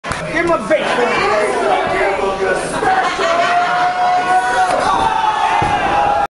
Exclusive Footage
Hurt shows LUW what a wrestler past his prime looks like.